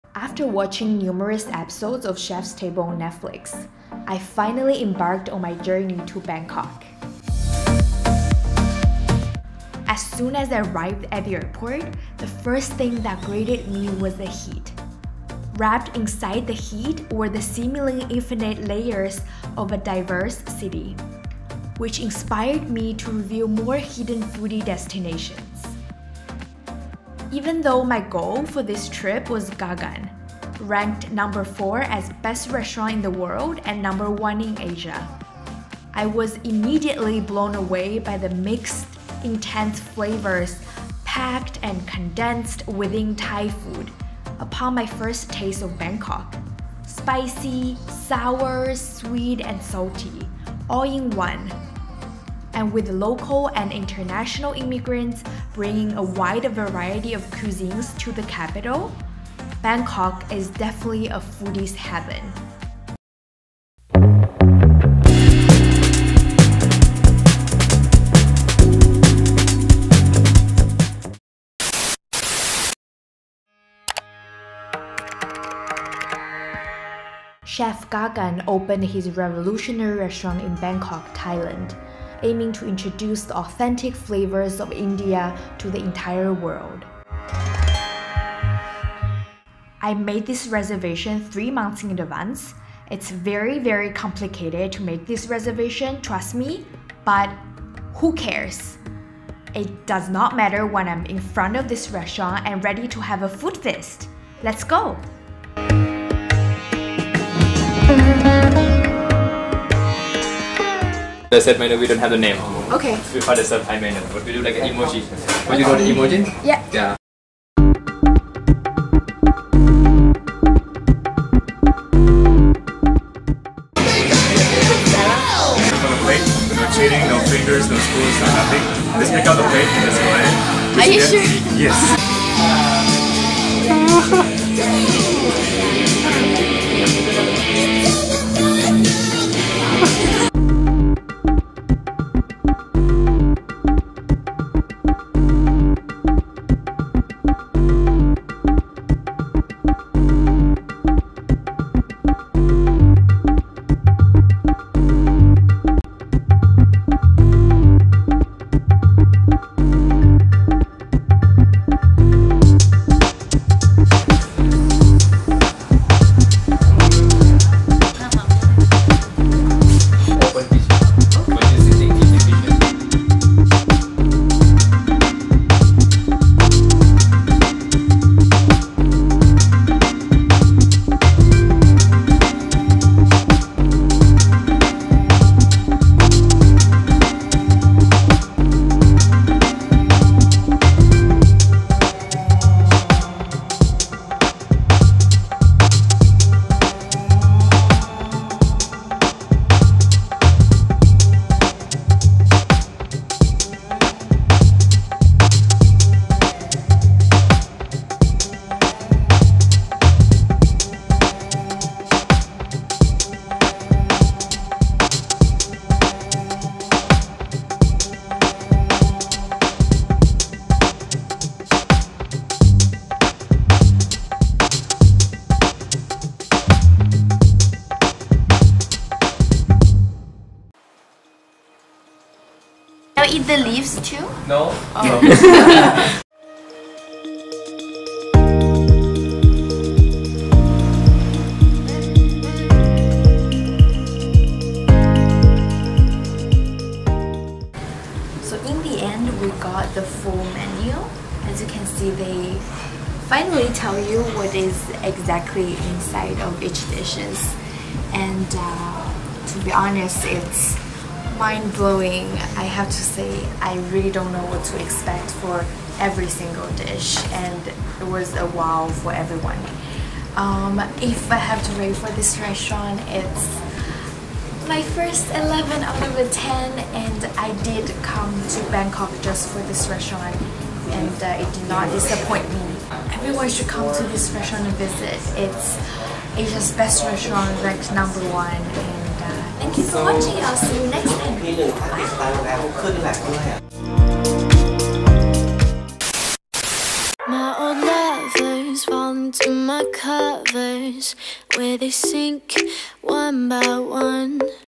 Gaggan | No.1 Best Restaurant in Asia & No.4 in the world | Bangkok, Thailand | Netflix Chef's Table
I visited Chef Gaggan Anand's restaurant "Gaggan" in Bangkok a while back, but the flavor explosion seemed to be fresh on my tongue while editing this video. 😋
I am very excited to share with you guys my dining experience. Even though Chef Gaggan has decided to close the restaurant, this memory will forever be close to my heart ❤️
Have a lovely weekend! 💗

Gaggan:
Address: 68/1 Soi Langsuan, Ploenchit Road, Lumpini, Bangkok 10330